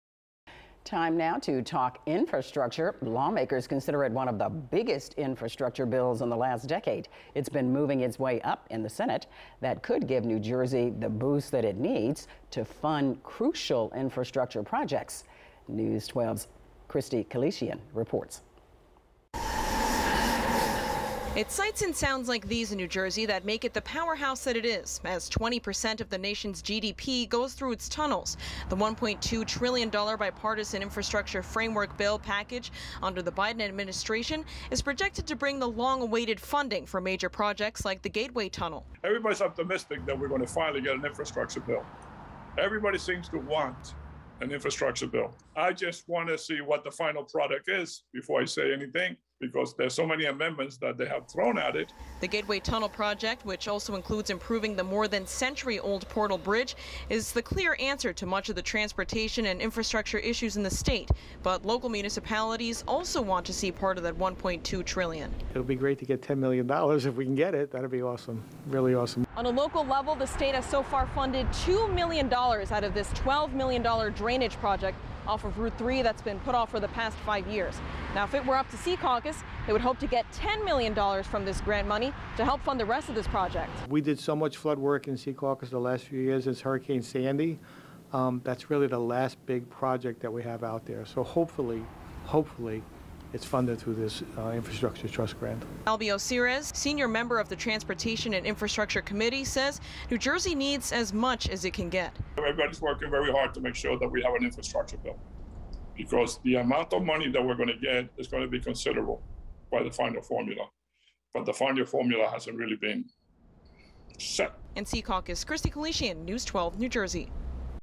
New Jersey leaders hopeful federal infrastructure bill will help fund state projects
A bill that lawmakers consider to be one of the biggest infrastructure bills in the last decade is moving its way up in the United States Senate.